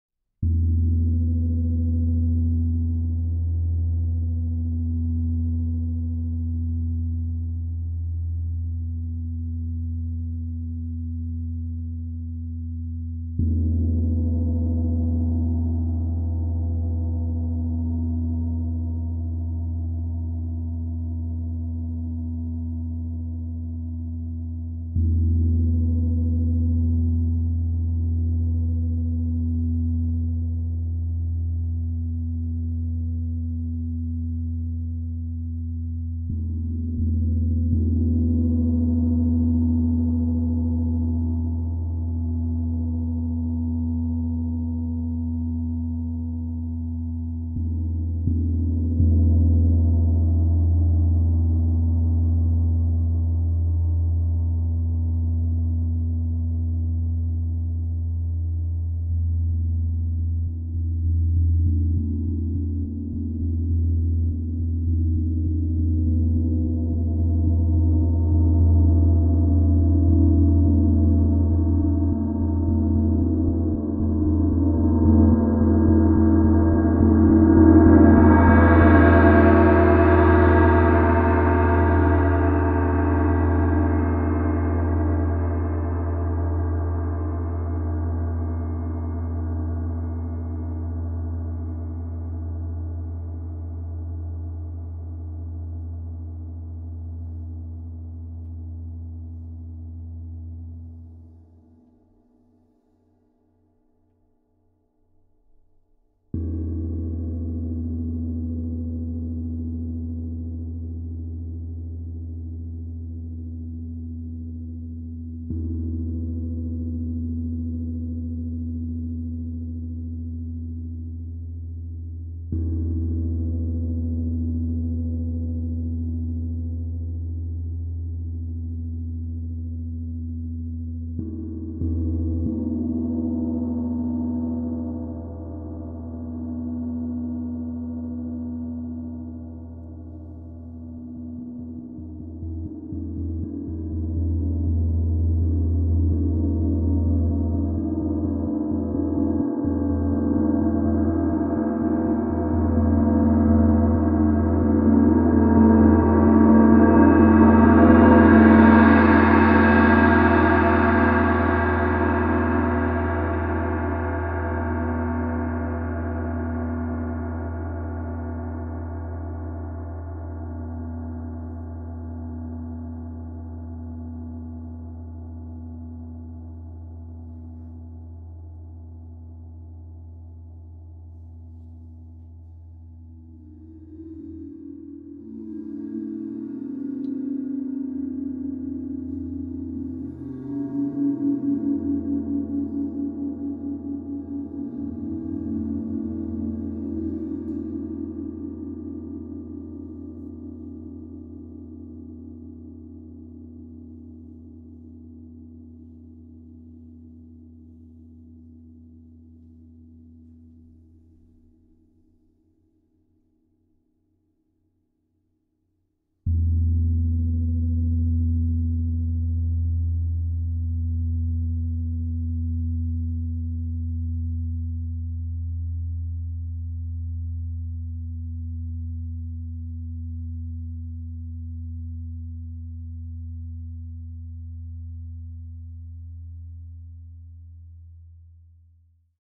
Paiste 30" Symphonic Gong - No Logo (SG15130-1081320J)
Paiste 30" Symphonic Gong - No Logo at myGong.com.

Other gear used in this video: Paiste M5, M12 Mallets, Mike Balter Super Rub Mallets